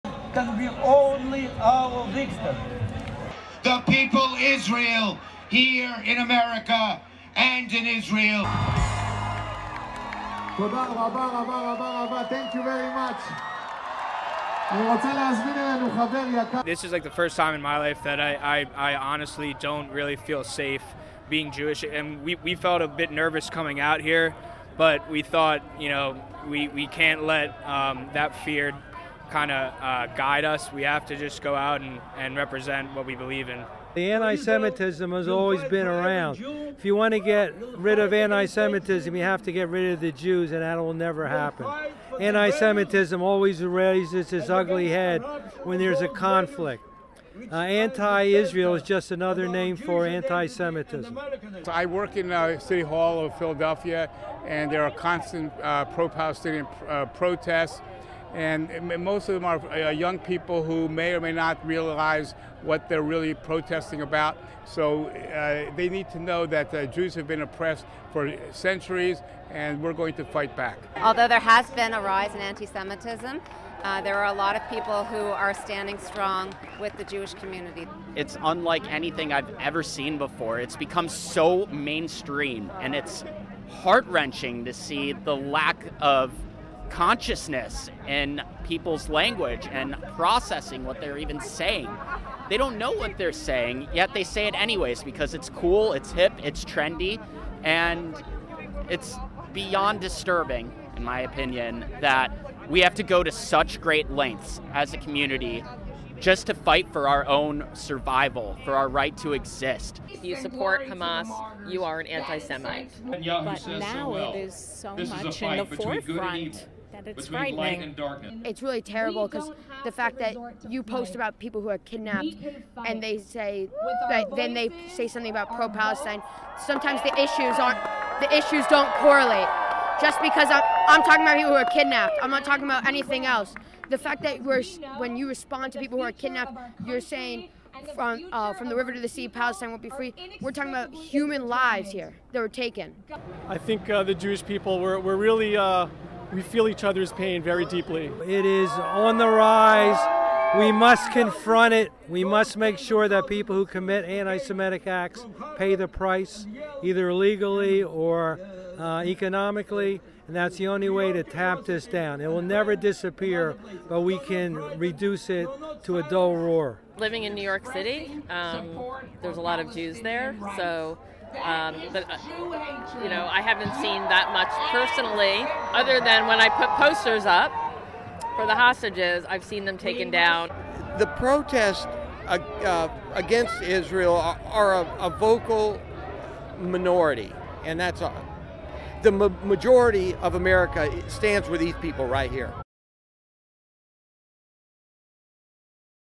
'Don't really feel safe': Pro-Israel activists detail antisemitic experiences
Taken down hostage missing posters, protests in the workplace and hateful posts online are just some of the ways pro-Israel demonstrators have experienced antisemitism.